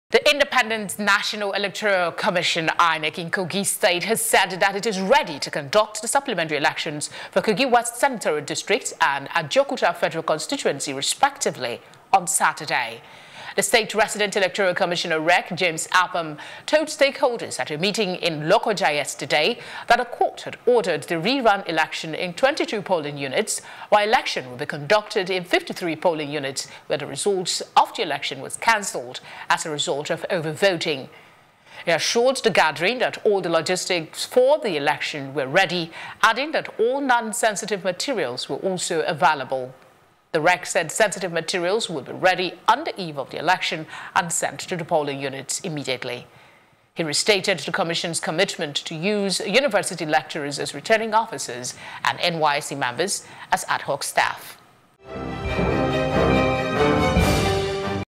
INEC Set For Supplementary Elections In Kogi
The Independent National Electoral Commission (INEC) in Kogi State has said that it is ready to conduct the supplementary elections for Kogi West senatorial district and Ajaokuta Federal Constituency respectively on Saturday.

The state Resident Electoral Commissioner (REC), James Apam, told stakeholders at a meeting yesterday in Lokoja that the court had ordered the rerun election in 22 polling units, while election would be conducted in 53 polling units where the result of election was cancelled as a result of over-voting.

He assured the gathering that all the logistics for the elections were ready, adding that all non-sensitive materials were also available.

The rec said sensitive materials would be ready on the eve of the election and sent to the polling units immediately.

He restated the commission’s commitment to use university lecturers as returning officers and NYSC members as ad hoc staff.